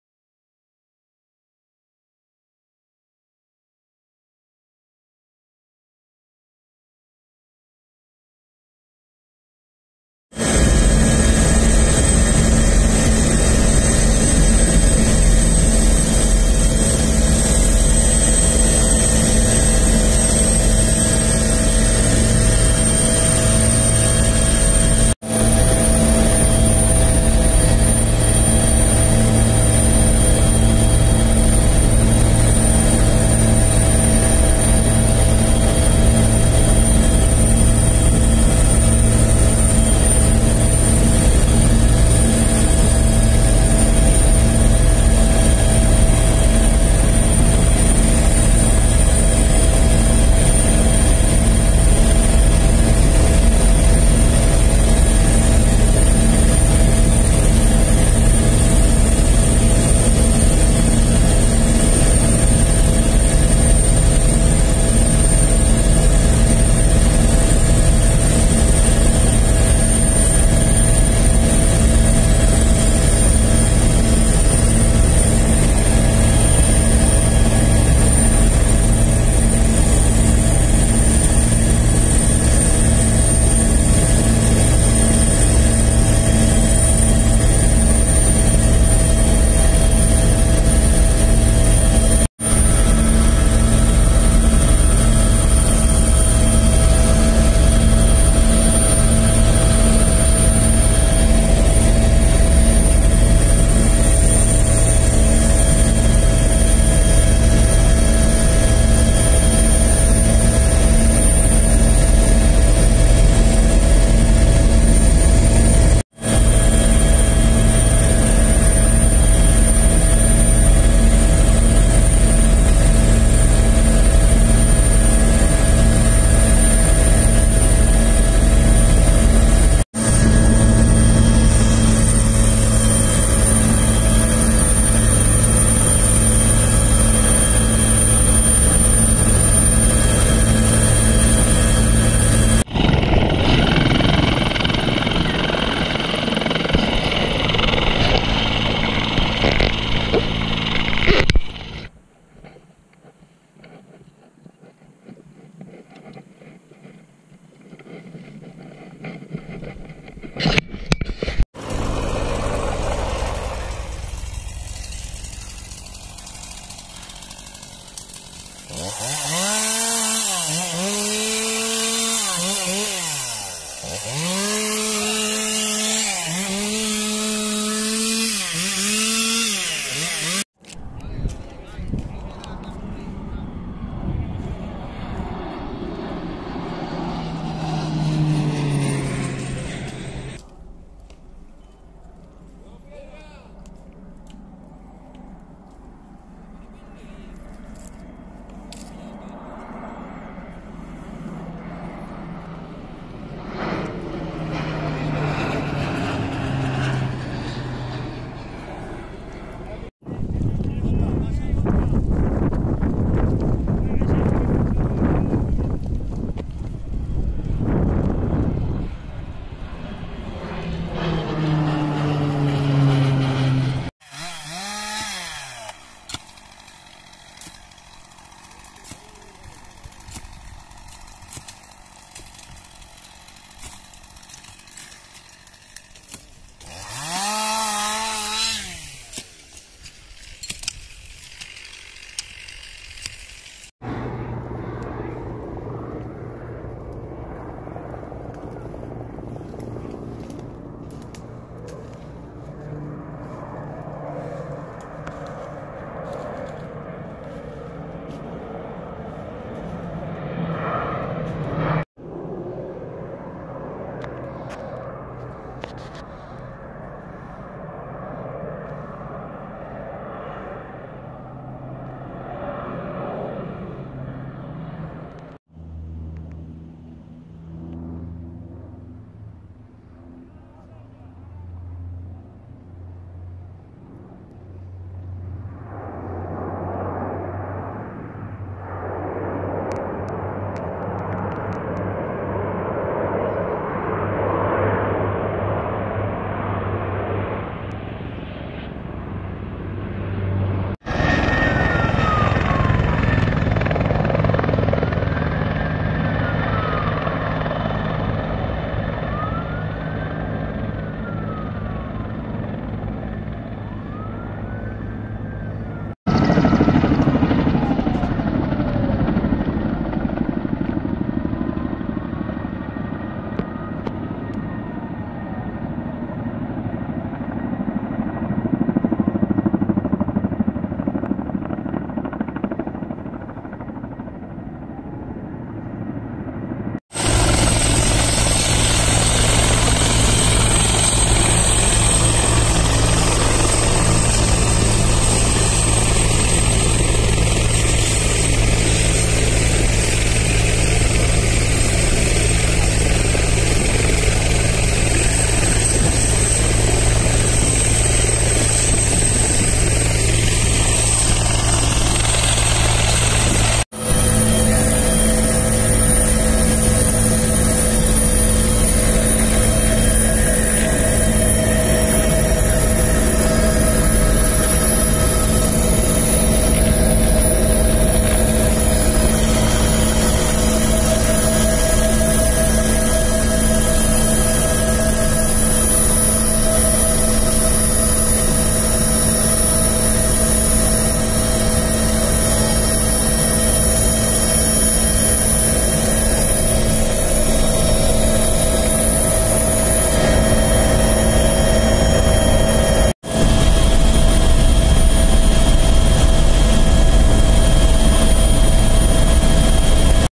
12-09-2020 IF Vilar de Barrio (BRIF Tabuyo)
Imágenes de la intervención de nuestros compañeros de BRIF Tabuyo (León) en el incendio forestal declarado en la localidad de Vilar de Barrio (Orense)